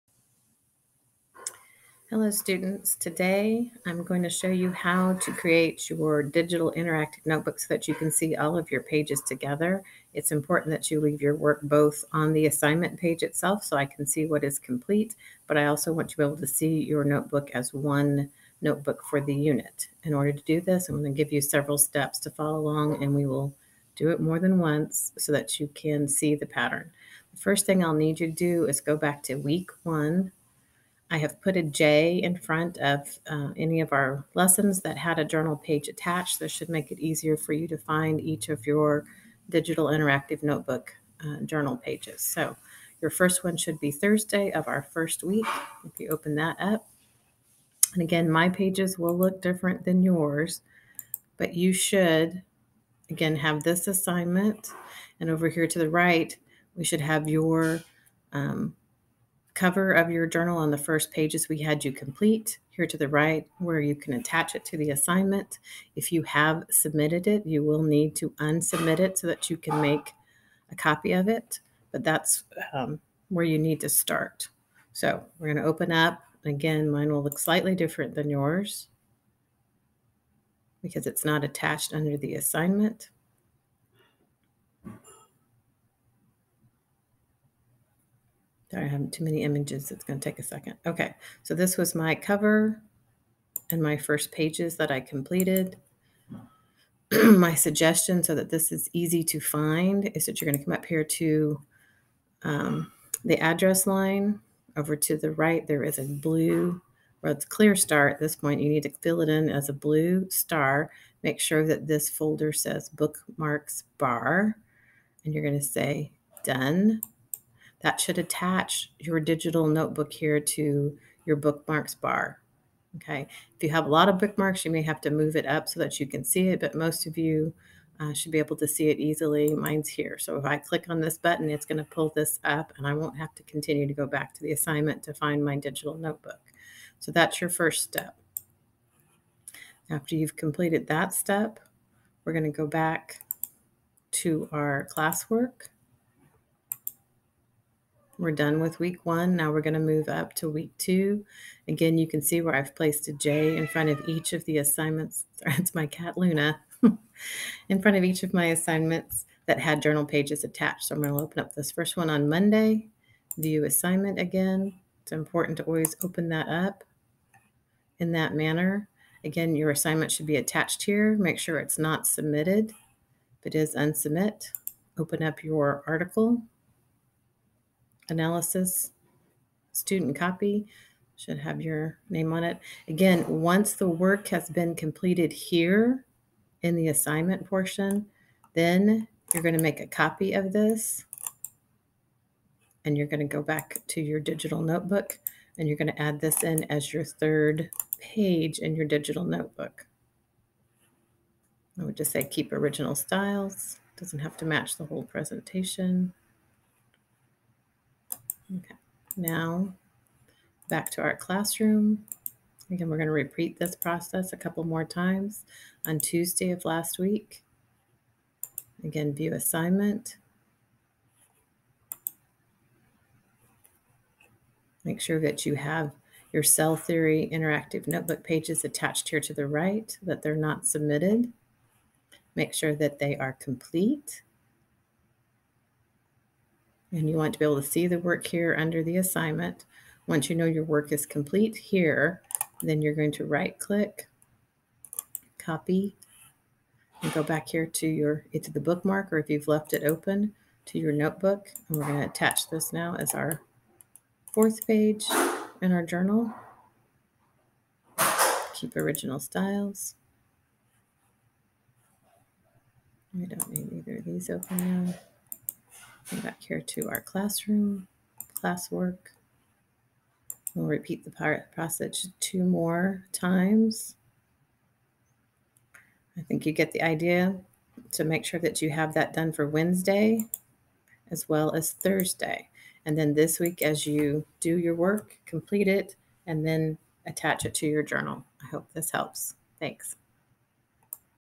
Digital Notebook Assembly: Sep 7, 2020 4:48 PM
Showing students how to assemble their digital notebooks